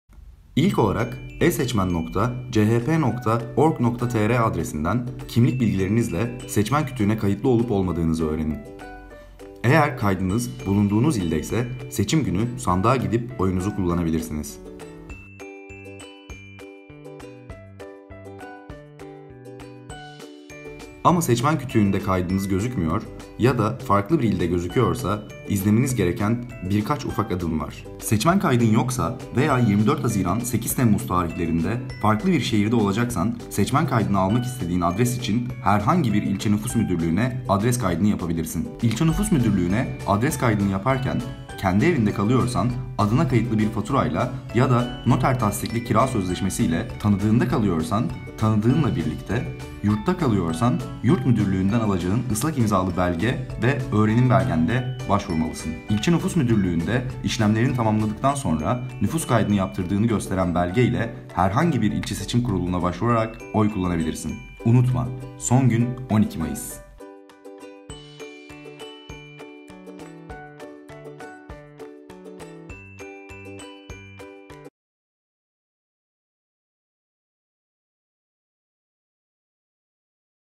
Seçmen Listeleri Askıda! Son Gün: 12 Mayıs
24 Haziran’da vatandaşlık görevini yerine getirmek için son gün 12 Mayıs! SeçmenMisin kontrol etmek için